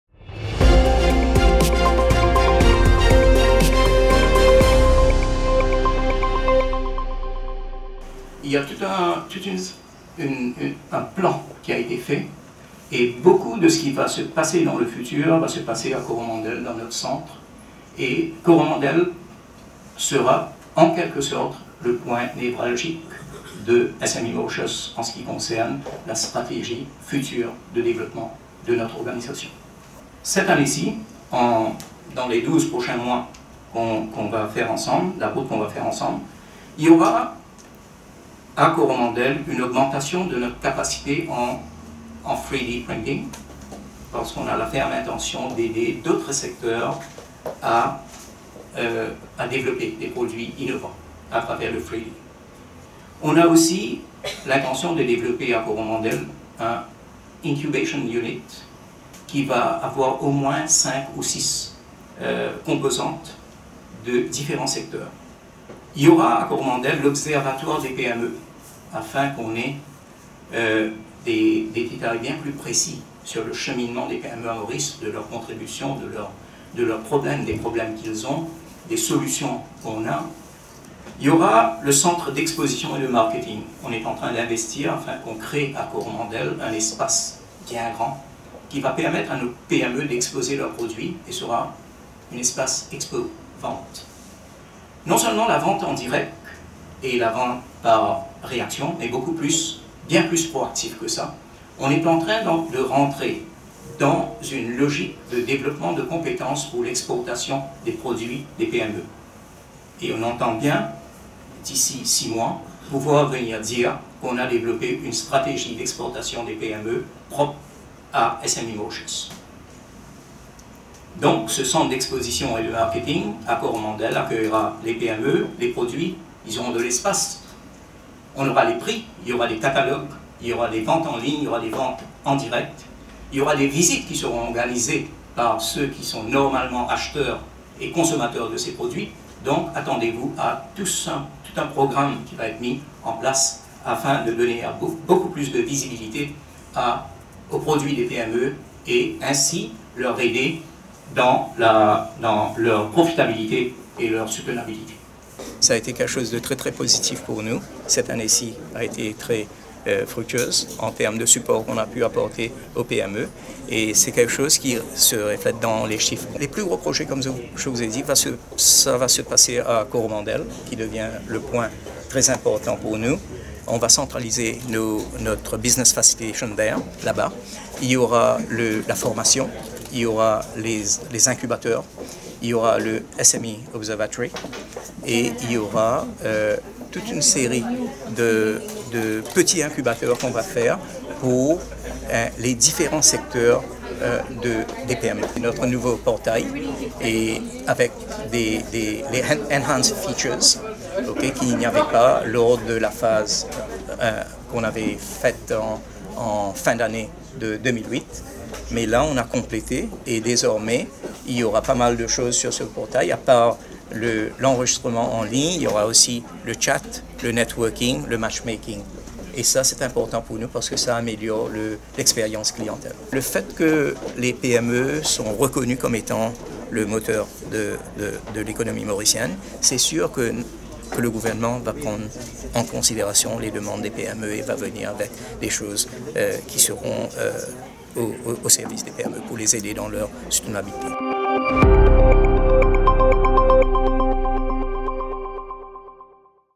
SME Mauritius concentrera plusieurs activités dans son centre de Coromandel
L’organisme Small and Medium Entreprises Mauritius (SME Mauritius) a dressé son bilan de l’année financière écoulé lors d’un point de presse, le vendredi 7 juin, au siège du SME Mauritius. Son CEO, Pravin Rampersad est satisfait des performances et des chiffres positifs pour la période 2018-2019.
La SME Mauritius souligne que le soutien apporté aux petites et moyennes entreprises (PME) ne s’arrêtera pas là. «Les plus gros projets seront centralisés à Coromandel», a expliqué Pravin Rampersad. Ce sera le centre important pour les PME. Les ncubateurs ou encore le SME Observatory y sont installés.
SME Mauritius prévoit aussi un espace d’expovente afin d’offrir plus de visibilité aux PME et à leurs produits.